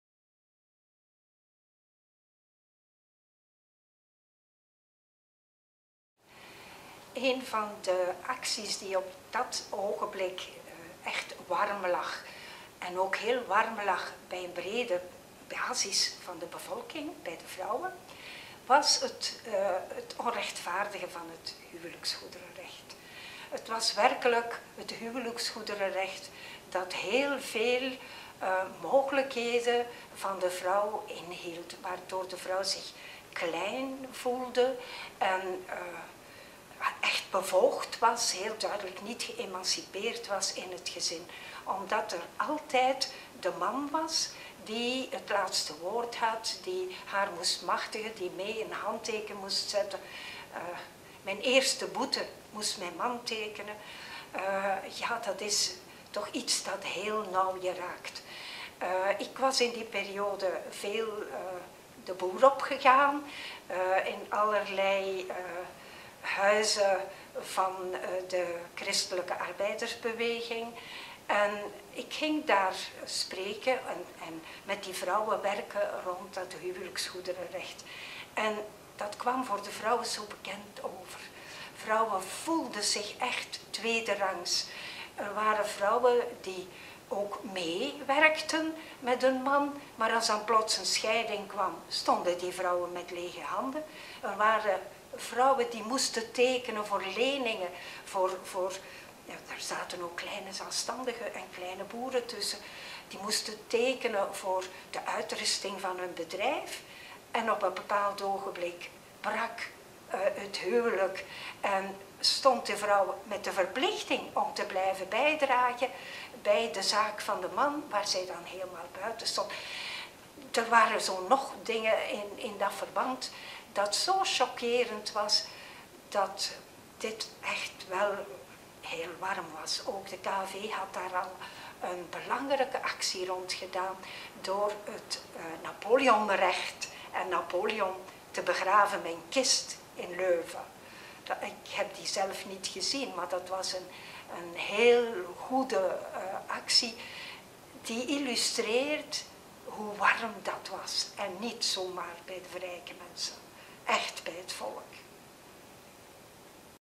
Rita Mulier: "Het huwelijksgoederenrecht"
De zwakke rechtspositie van de gehuwde vrouw was eind 19de eeuw al een strijdpunt voor de vrouwenbeweging. Halfweg de jaren 1970 was er nog één kwestie te regelen: gelijkheid in het beheer van de 'huwelijksgoederen'. Rita Mulier vertelt er meer over.